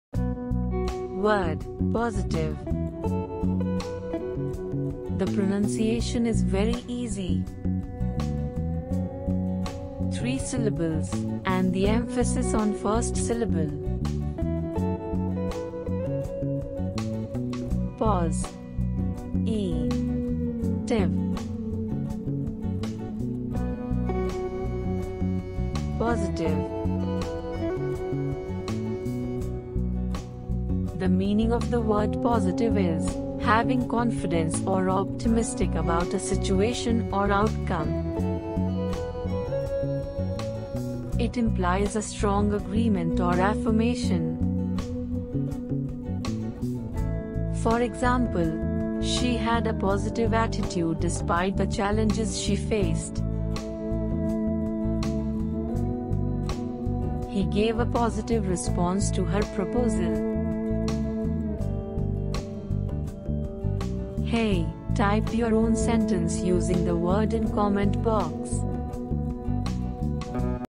Tool to Remember Forever : English Word "POSITIVE" | Vocabulary
In this video, we'll be learning the English word "POSITIVE"   We'll be using the Visual Example Tool to help us remember this word.

In the next video tomorrow, we'll be learning the English word "OBSERVANT"

Expand you Word Vocabulary with Examples

Explore other learning videos through series of playlists mentioned below.